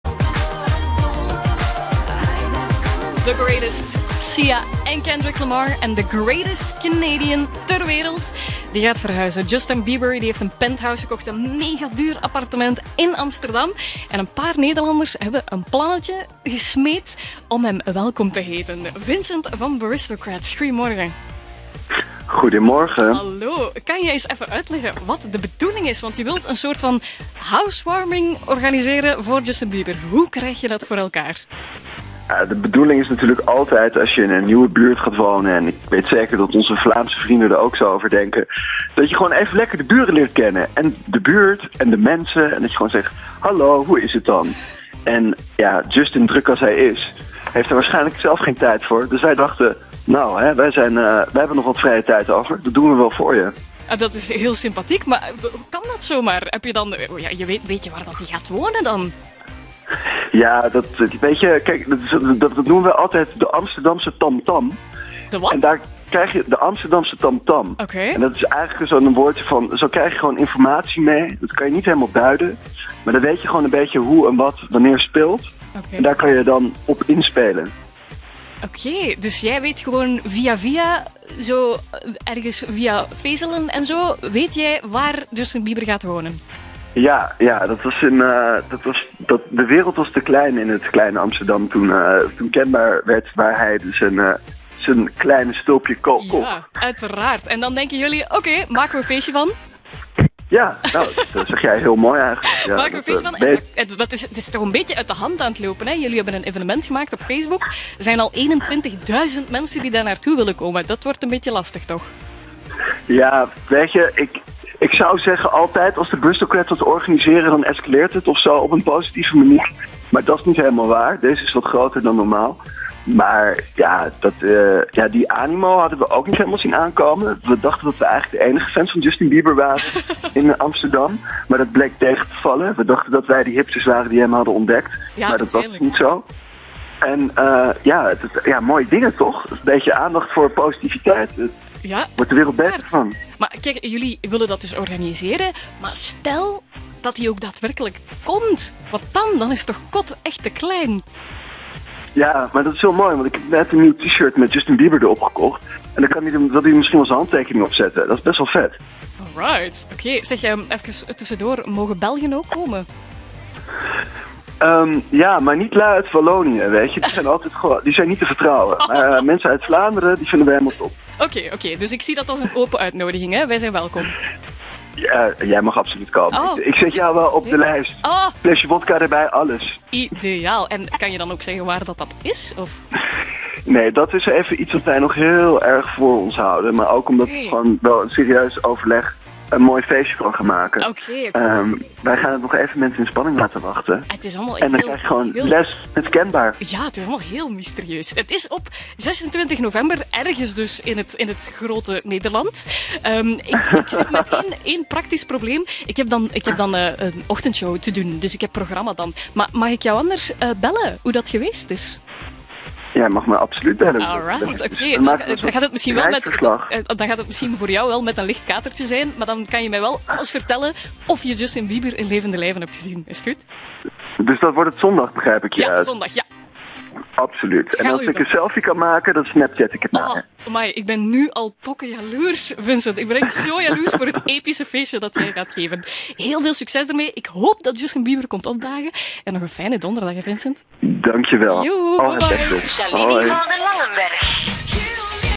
Ga jij naar de housewarming van Justin Bieber?
Justin Bieber heeft een appartement gekocht in Amsterdam. Toen dat nieuws bekend raakte, was Amsterdam te klein. Shalini belde met Vincent, die samen met enkele vrienden een housewarming voor Bieber organiseert. Of Justin zelf ook komt opdagen, is nog af te wachten...